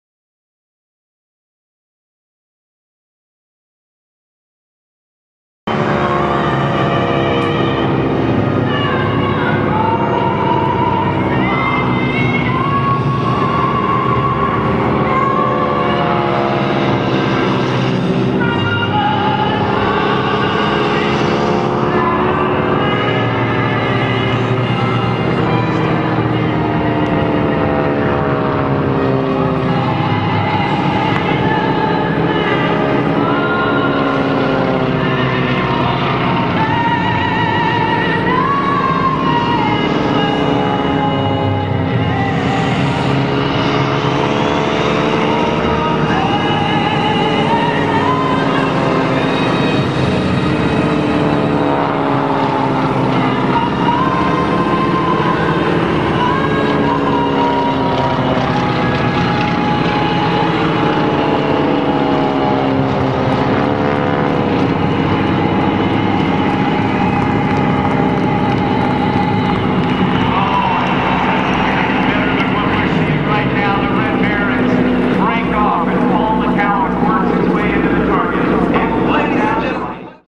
The Gathering of Mustangs and Legends 2007 Airshow Opening Ceremony
The United States flag and the Red Baron Squadron performing the opening ceremony at The Gathering of Mustangs and Legends in Columbus, Ohio in September 2007.